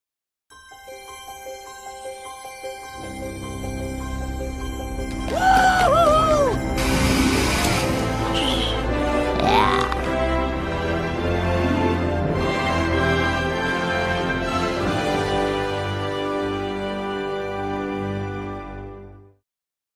MediaToon Cartoon Studios 20th Anniversary Logo (2012)
This logo debuted in 2012 as part of its 20th Anniversary.

First Seen in: The Lorax (2012; NATAD AU)
Last Seen in: Rise of the Guardians (2012; NATAD AU)